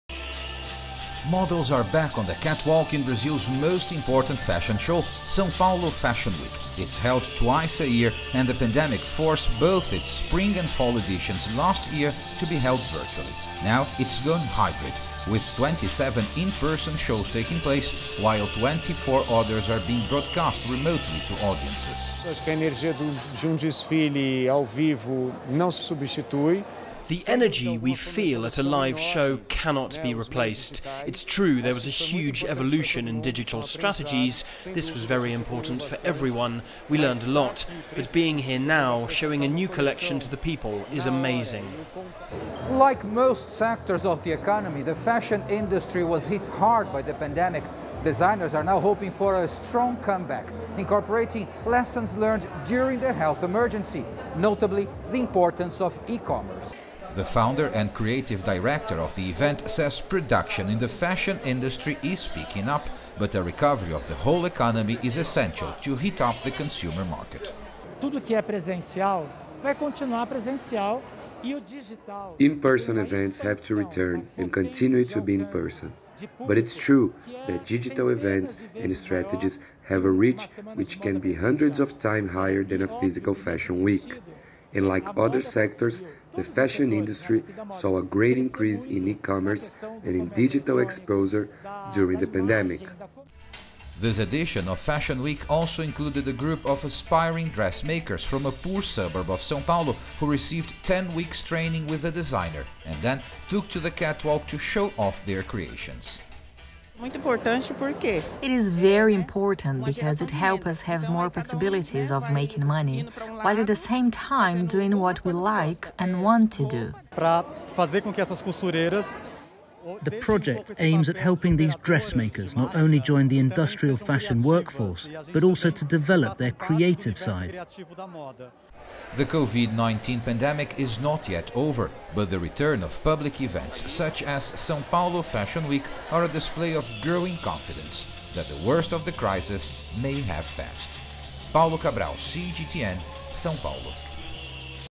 Sao Paulo holds first in-person Fashion Week since pandemic
Brazil's main fashion show São Paulo Fashion Week is back to an in-person format after two virtual shows during the pandemic. Hopes are that this signals a comeback for the fashion industry. CGTN's Paul Cabral reports.